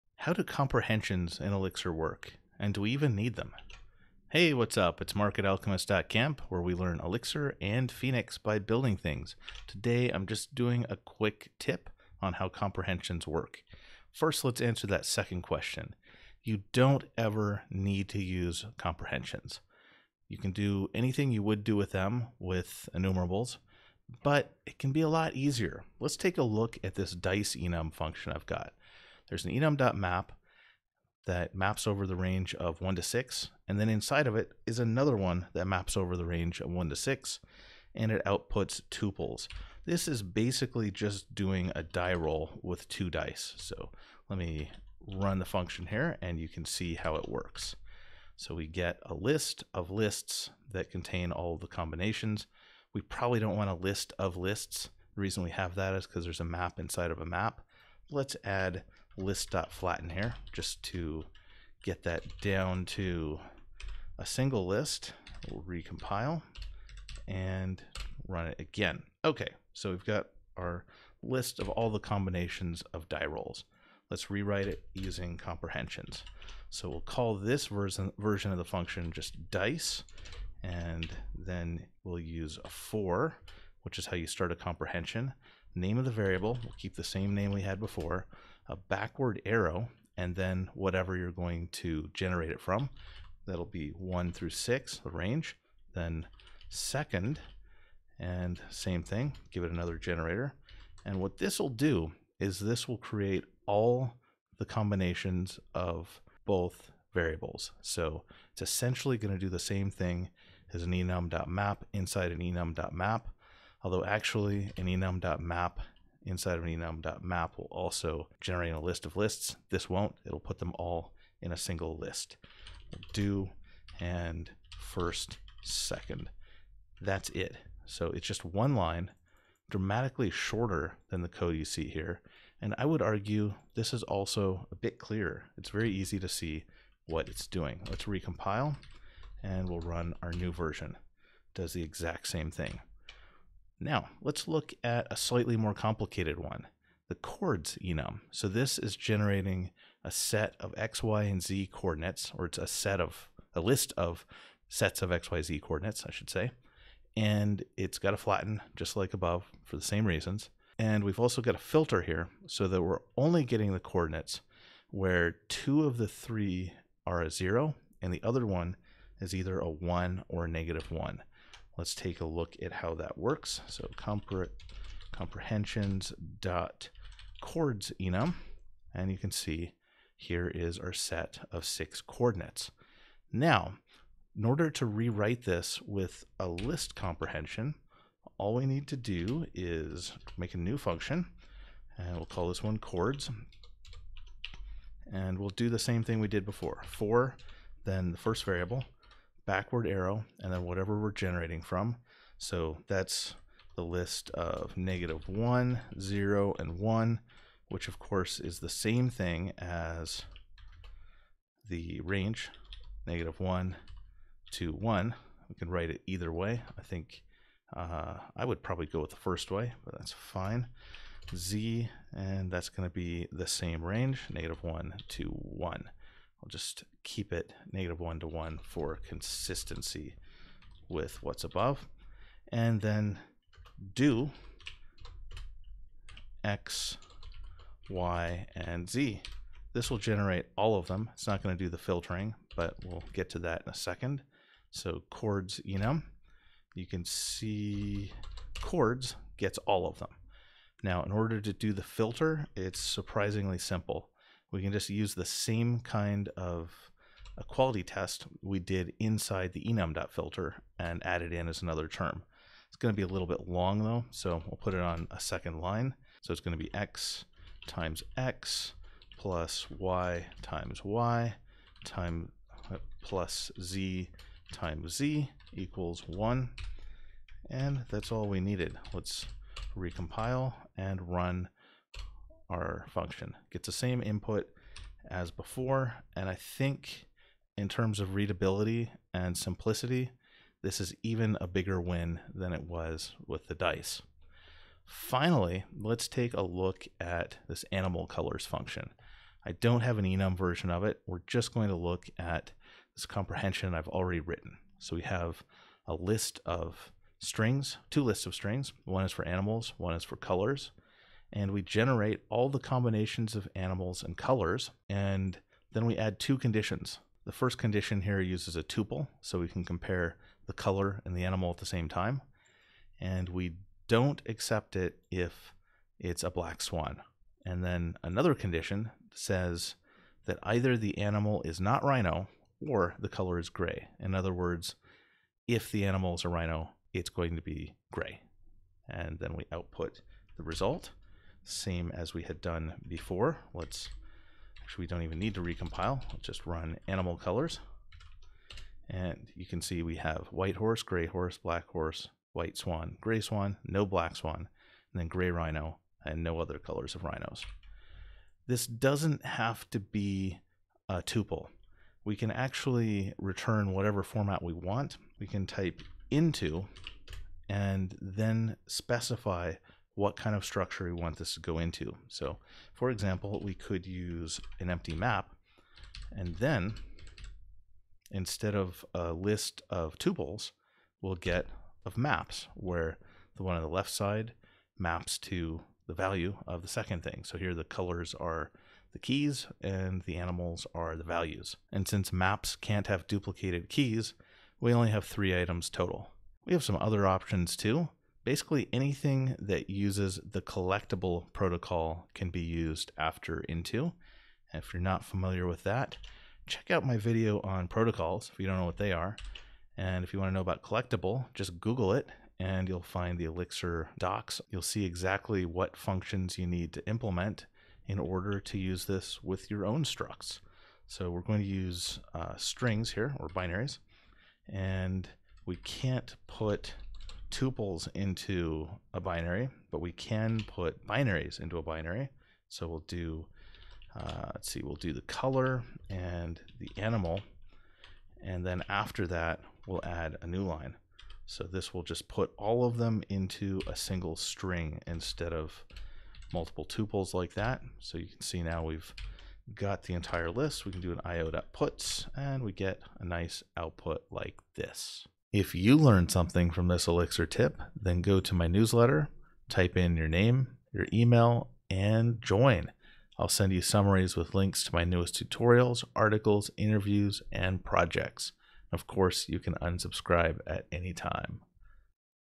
How do comprehensions work in Elixir?
Comprehensions in Elixir are terse and powerful. Let's look at where they're useful, how to write them and how to tweak them.